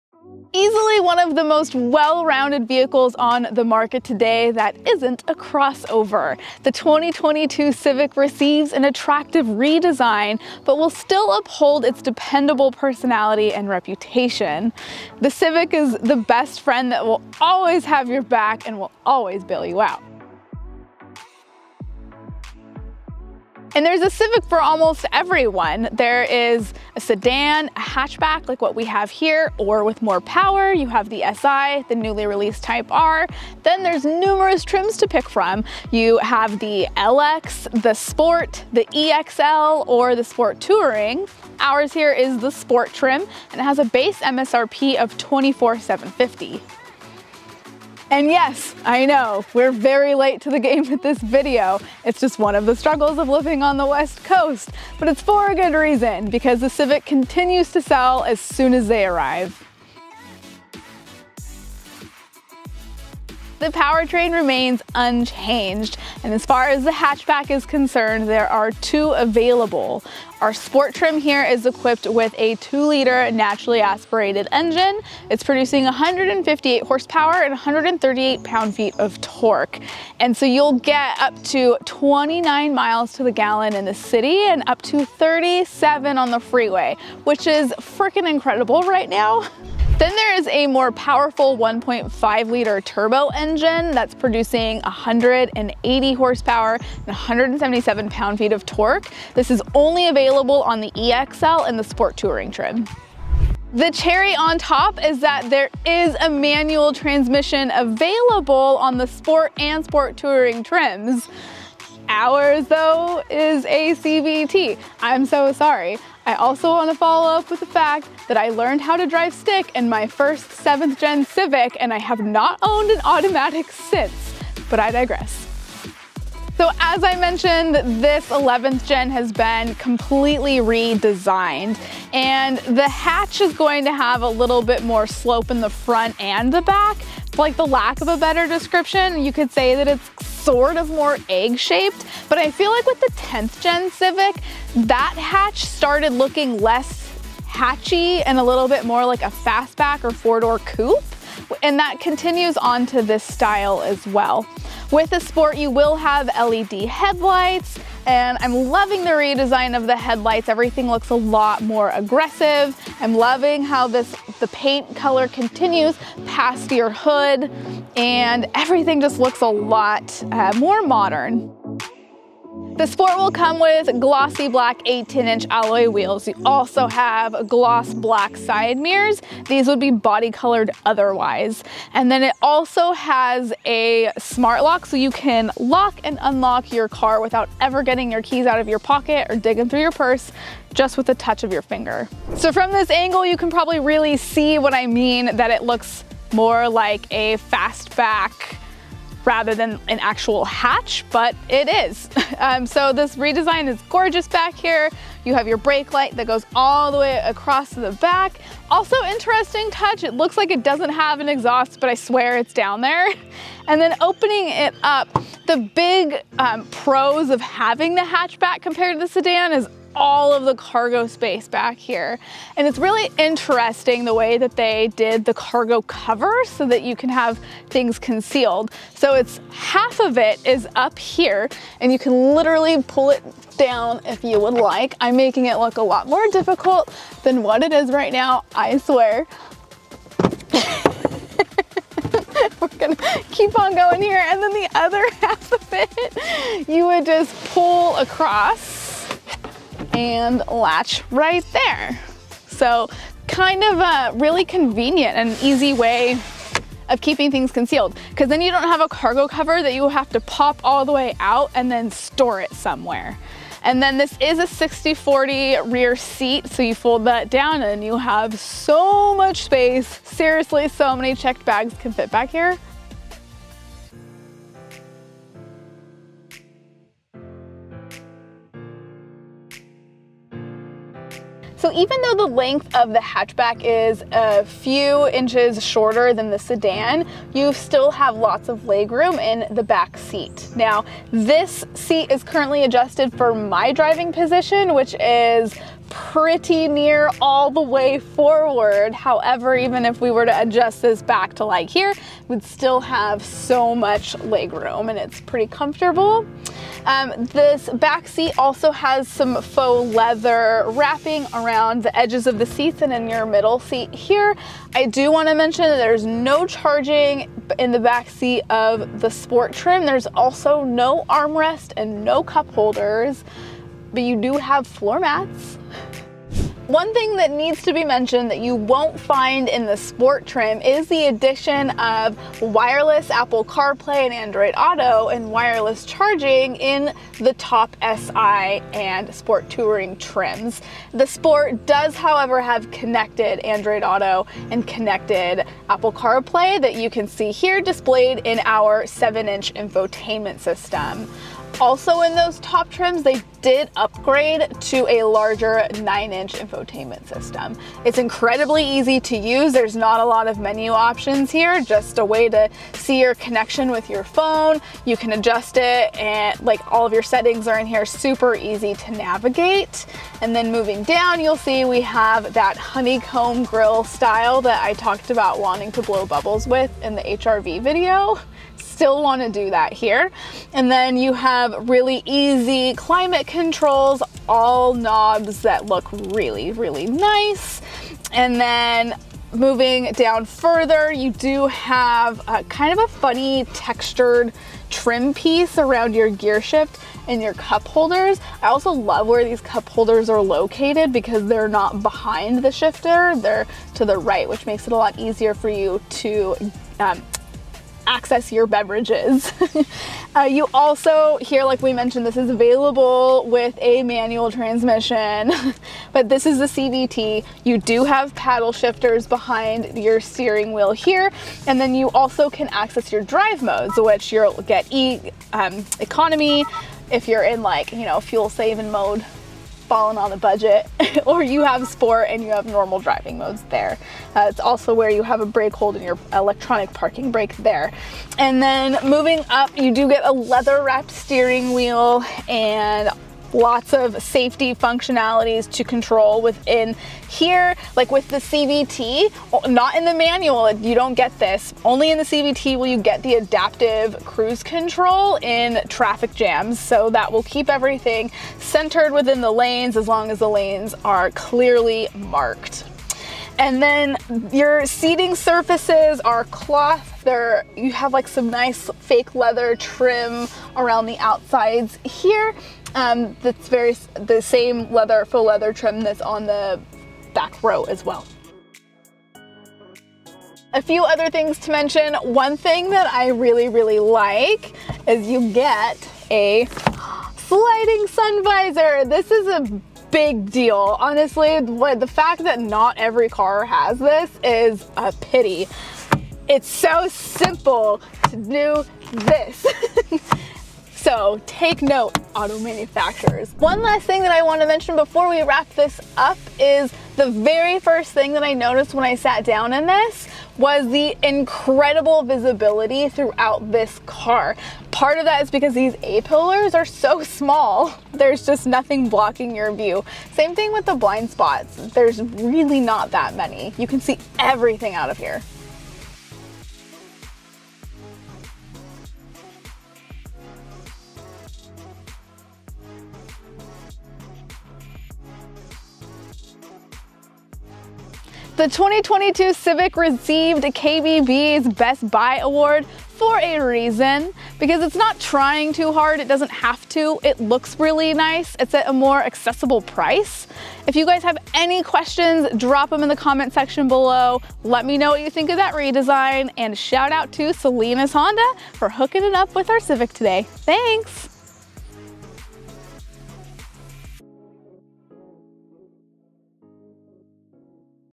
2022 Honda Civic Hatchback Sport Review, Never Gonna' Let You Down!
You know whenever you get into a Honda Civic you're going to get quality, reliability, affordability and comfort. The 2022 Honda Civic Hatchback Sport is no exception! Don't let the redesign fool you, though it might have a more sophisticated look, it still delivers the same reliable ride that made you fall in love with it the first time. Check out what the 2022 model has to offer and let us know what you think about this new look!

0:00 Intro
1:03 Powertrain
1:19 Mileage
2:06 Redesign
2:31 Ext. Features
3:35 Trunk Space
4:41 Back Seat
5:26 Front Seat
8:00 Notable Mentions
8:57 Outro